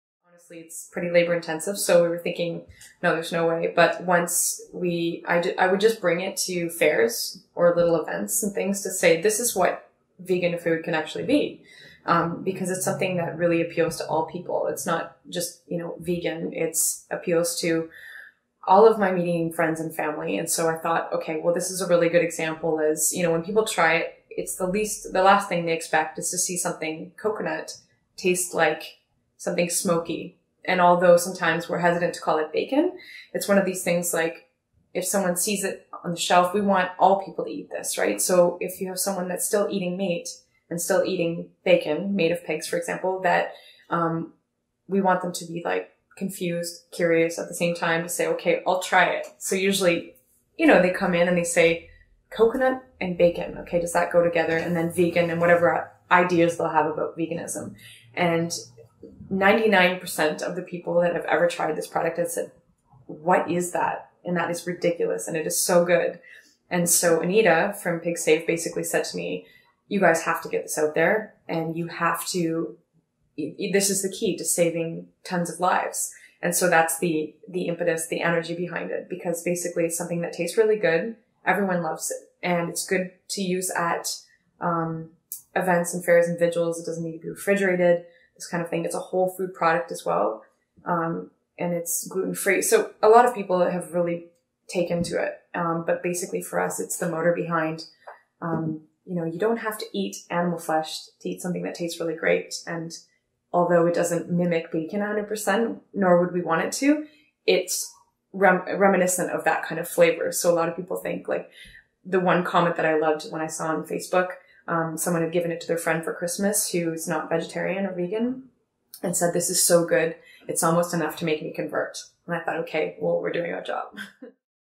Kristen Bethel Talking about Vegan Coconut Bacon
Someone I've met recently and will probably be doing some work with in the future. She's a yoga instructor, vegan cooking instructor, and she has a few other tricks up her sleeve planned for the future. If you're looking to learn about vegan cooking in the Toronto area, I definitely recommend her.